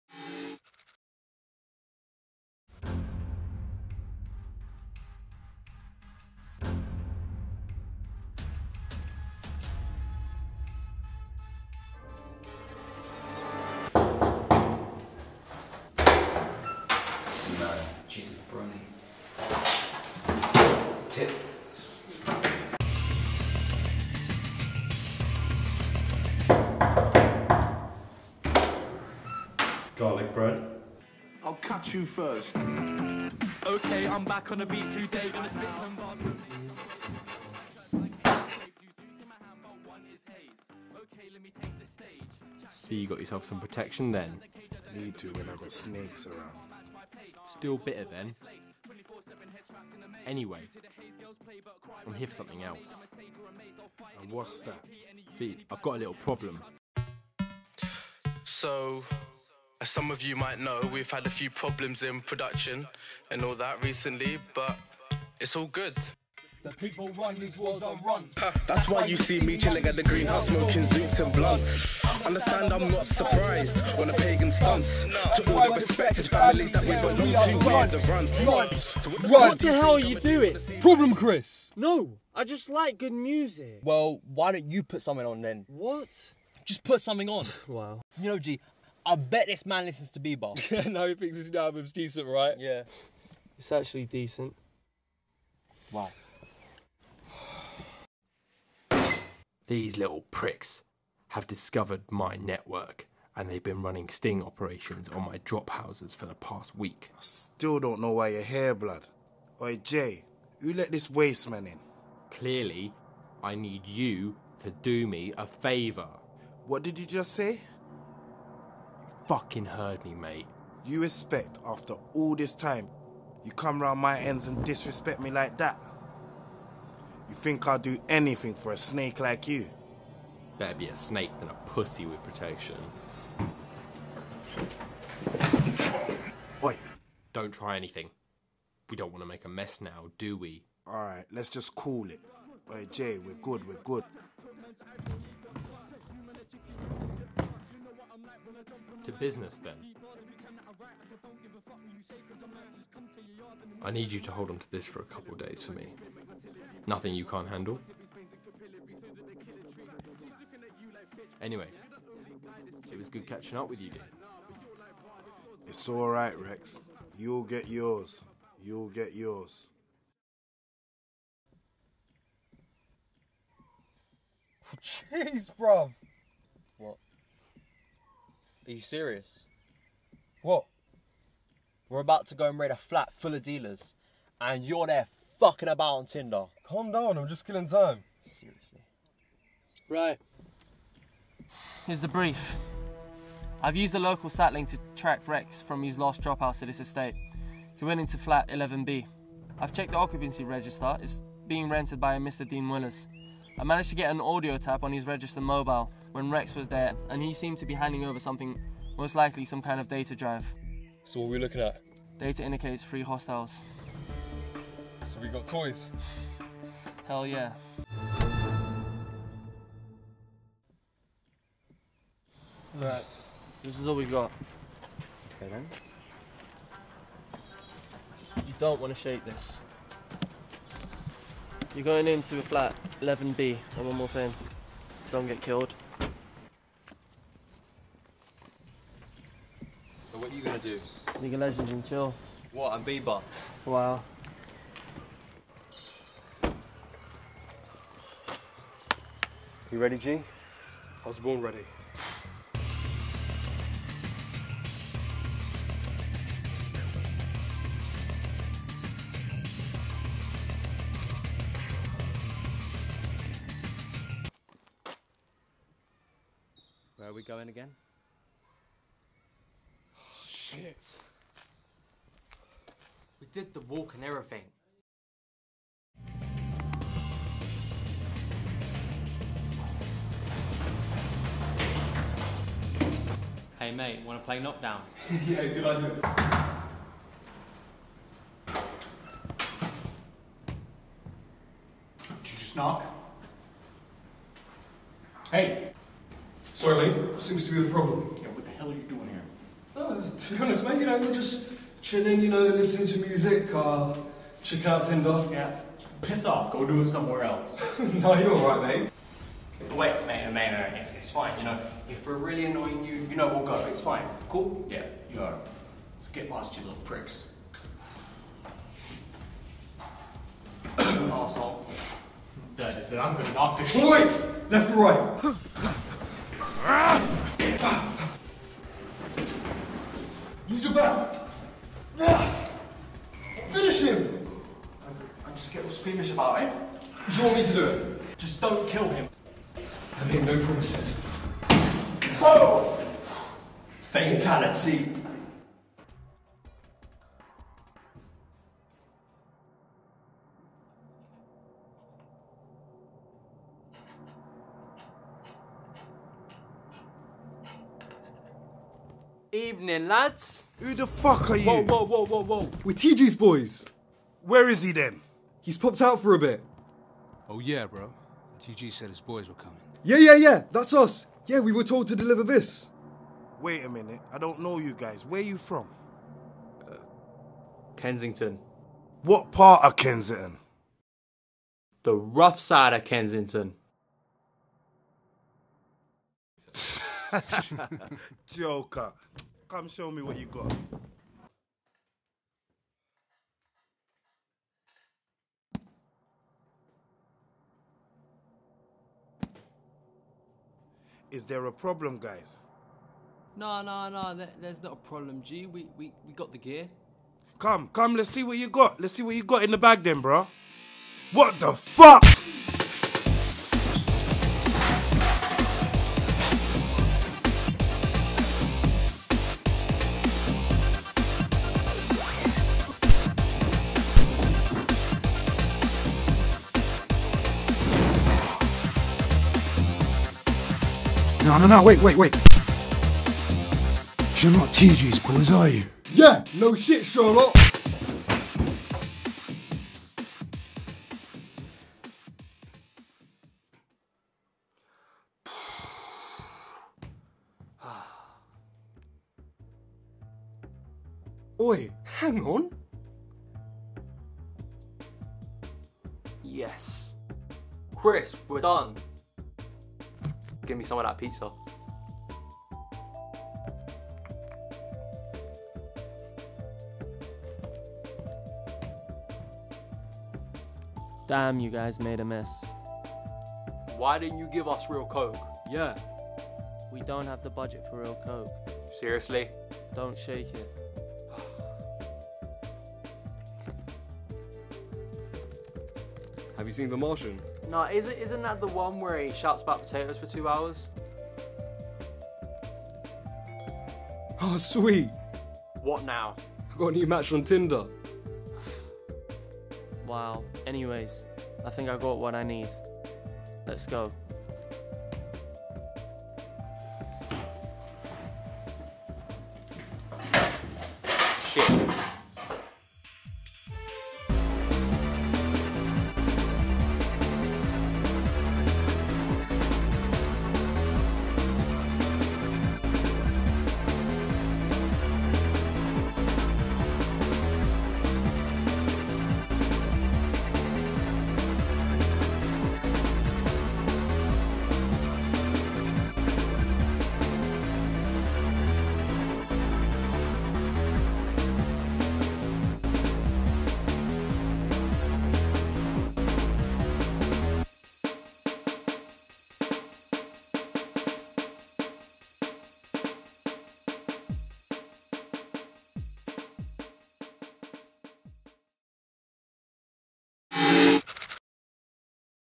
Another Spy Film (Short Film)
A small group of rogue operatives take it upon themselves to try and stop one of London's biggest Criminal Networks.

Written & Directed by Daniel Childs

 Cast

Pizza Boy - Isaac Quinn
"TG" Bodyguard - Trevor Zelch
Dean Willers - Corey Simms
Jay - Francisco Javier Ortiz
Matt - Micheal Jassey
Will - Daniel Childs
Chris - Miguel Pires

Audio:
Rania Beniich
Miguel Pires
Isaac Quinn

Camera:
Miguel Pires

Massive thanks to GHG for allowing us use of their music for this production. Also thanks to close friends and family who graciously allowed us to use their properties for filming.

Lastly, a MASSIVE thank you to everyone who was a part of this project including the entire cast & crew who made this film a reality.

All the characters, content, storylines & plot contained within this production are Copyright Property 2016 of Red Unit Production. The script which is based on the story created by Daniel Childs are Copyright Property 2016 of Daniel Childs.